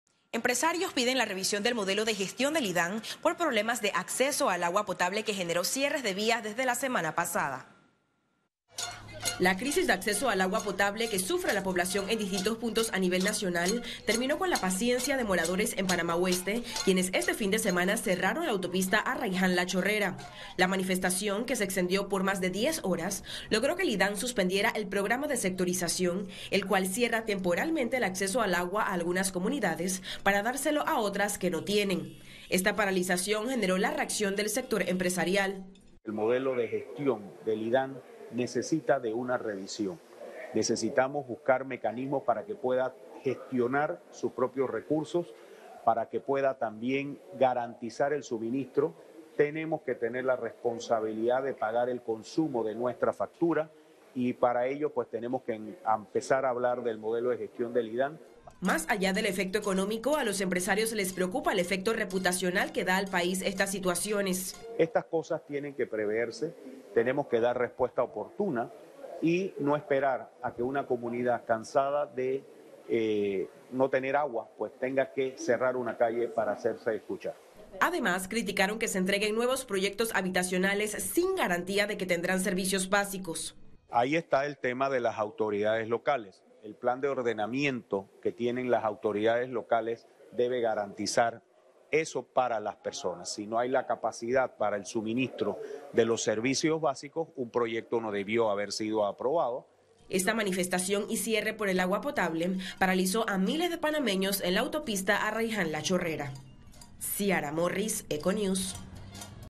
Empresarios piden evaluar modelo de gestión del IDAAN
Panamá, 03 de abril de 2023

Información por: Ciara Morris

Empresarios piden la revisión del modelo de gestión del IDAAN por problemas de acceso al agua potable que generó cierres de vías desde la semana pasada.

La crisis de acceso al agua potable que sufre la población en distintos puntos a nivel nacional, terminó con la paciencia de moradores en Panamá Oeste, quienes este fin de semana cerraron la Autopista Arraiján-La Chorrera.

La manifestación que se extendió por más de 10 horas logró que el IDAAN suspendiera el programa de sectorización, el cual cierra temporalmente el acceso al agua a algunas comunidades para dárselo a otras que no tienen.

Esta paralización generó la reacción del sector empresarial.

Más allá del efecto económico, a los empresarios les preocupa el efecto reputacional que da al país estas situaciones.

Además, criticaron que se entreguen nuevos proyectos habitacionales sin garantía de que tendrán servicios básicos.